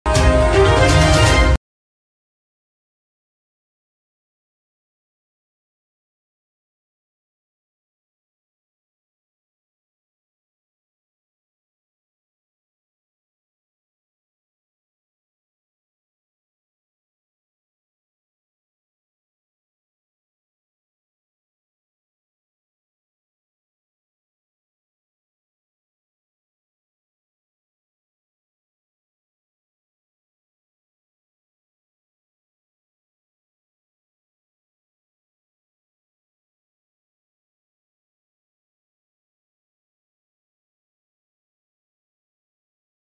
Punjab Govt Action Against Sugar Mills Owner | Sugar Mill Owner ko NOC Dynay Sy Inkar | South Today
Sugar mill owners threaten not to start crushing if NOC is not received | Punjab Govt ki Sugar Mills ke khilaf Kaarwai